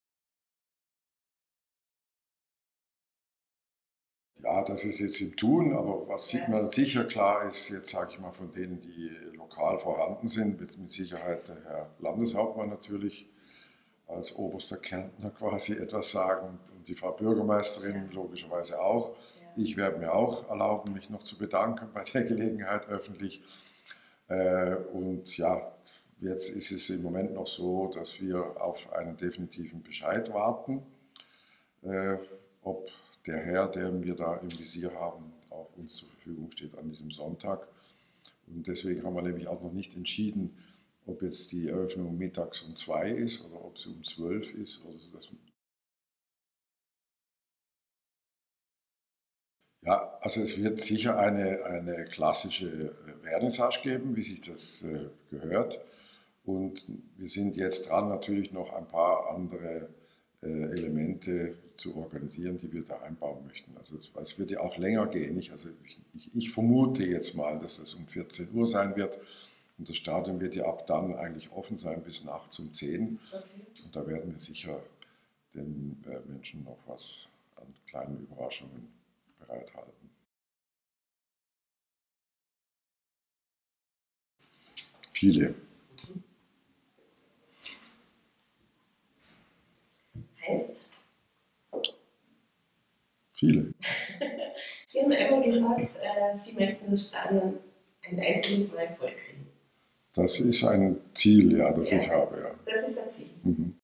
Teil 22
Bis zum Start des aufseheneregenden Kunstprojektes „For Forest“ im September präsentiert die „Kärntner Krone“ jede Woche die wichtigsten Fragen und Antworten rund um den Wald im Stadion. Initiator und Kunstvermittler Klaus Littmann erteilt Auskunft über Österreichs größtes Kunstprojekt in Klagenfurt.

Copyright bitte „Clara Milena Steiner (Kamera & Schnitt)“.